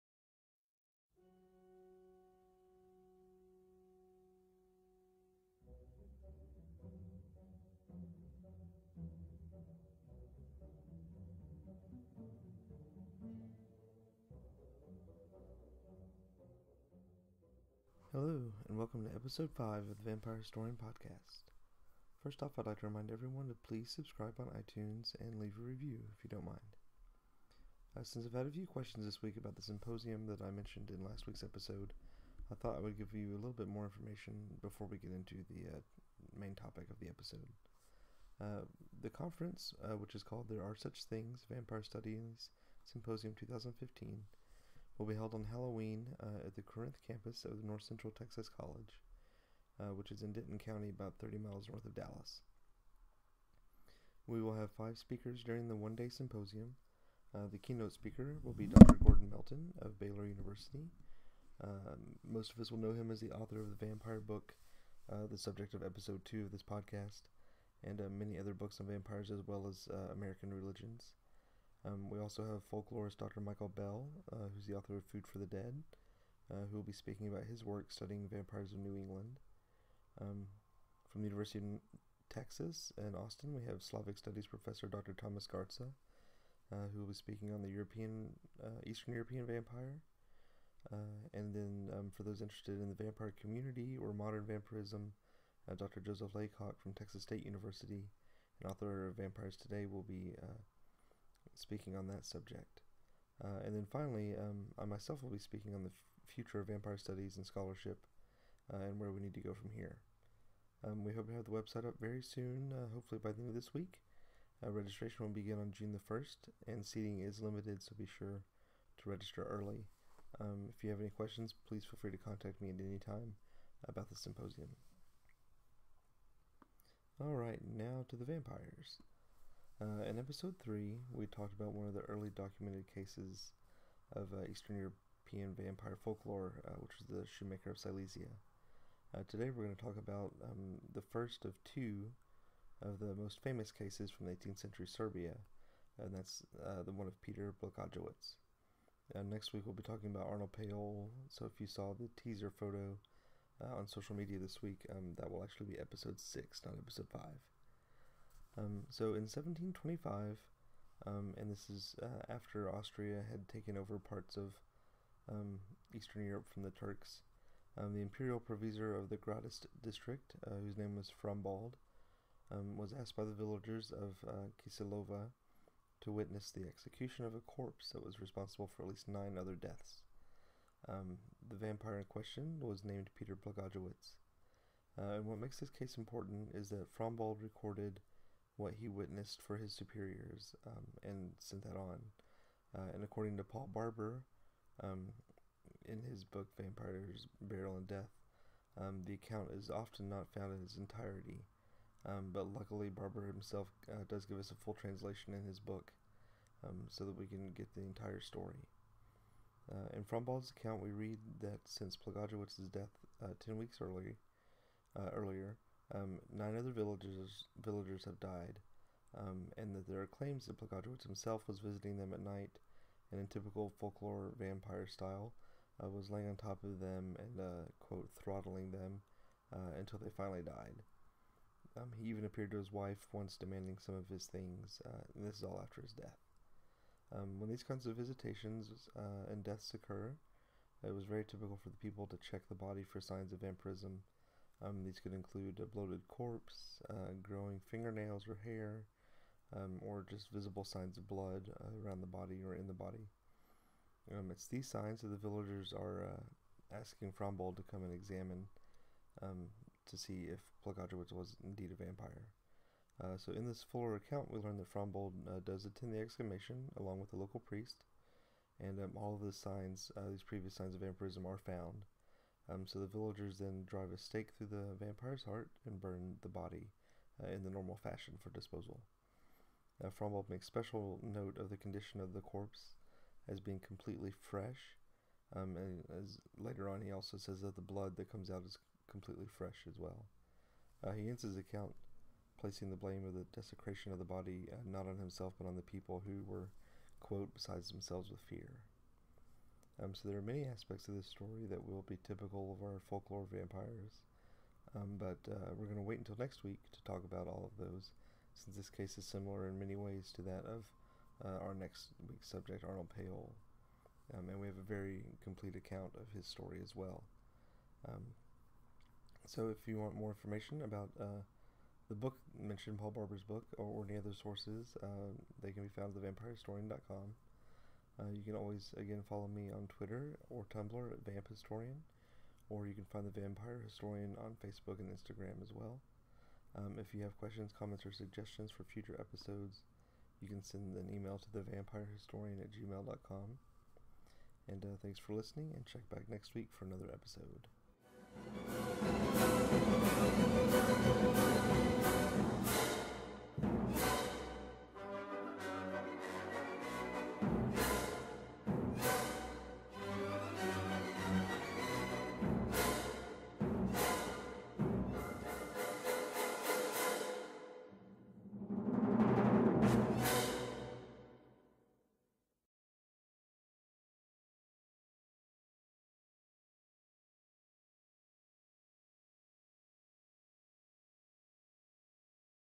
The Vampire Historian Podcast, Episode 5: Austria Meets the Vampires
In Episode 5, I talk about the first of 2 major historical vampires from the Austrian empire in the 18th century: Peter Plogojwitz.